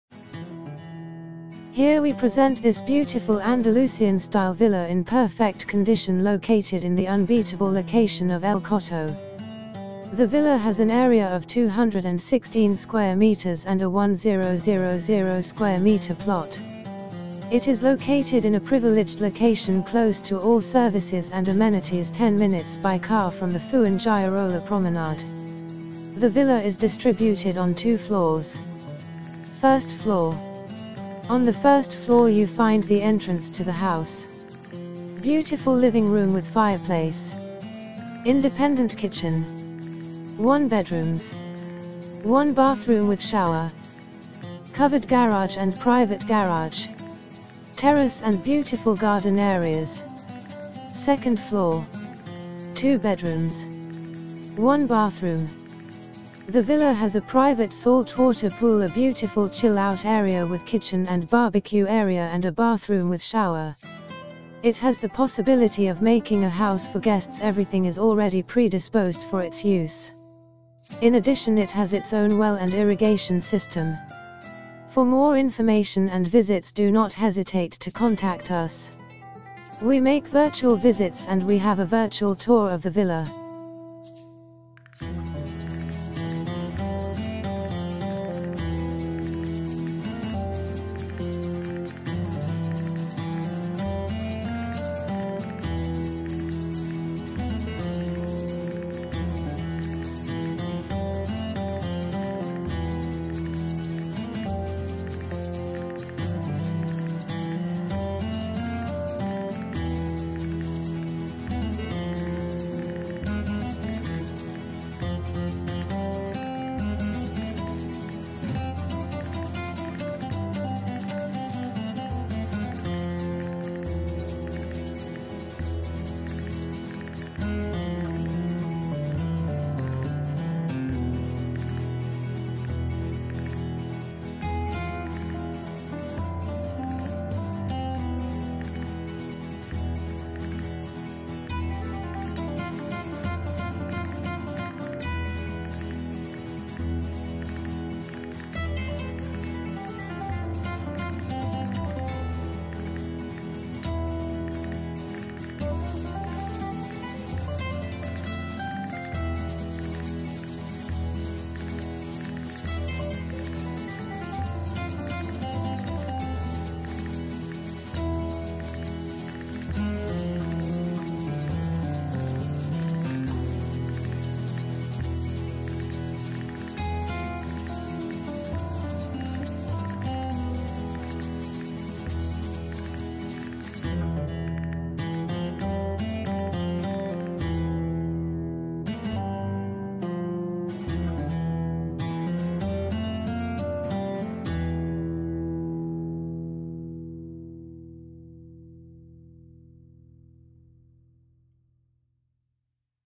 3 Bedrooms Detached Villa in El Coto, Costa Del Sol FOR SALE €739,000
3 Bedrooms Detached Villa in El Coto, FOR SALE €739,000 Property ID: CDS15973
Here we present this beautiful Andalusian-style Villa, in perfect condition, located in the unbeatable location of El Coto.

The villa has an area of 216 square meters and a 1,000 square meter plot,
It is located in a privileged location, close to all services and amenities, 10 minutes by car from the Fuengirola promenade.

The villa is distributed on two floors:
First floor:
On the first floor, you find the entrance to the house
Beautiful living room with fireplace
Independent kitchen.
1 bedrooms.
1 bathroom with shower
Covered garage and private garage.
Terrace and beautiful garden areas.

Second floor
2 bedrooms
1 bathroom

The villa has a private saltwater pool, a beautiful chill out area with kitchen and BBQ area and a bathroom with shower.
It has the possibility of making a house for guests, everything is already predisposed for its use.

In addition, it has its own well and irrigation system.
We make virtual visits and we have a virtual tour of the villa.Covered Terrace;Fitted Wardrobes;Near Transport;Private Terrace;ADSL/WIFI;Guest Apartment;Utility Room;Staff Accommodation;Near Church;Fiber OpticTown;Beachside;Close To Shops;Close To Sea;Close To Town;Close To Schools;Urbanisation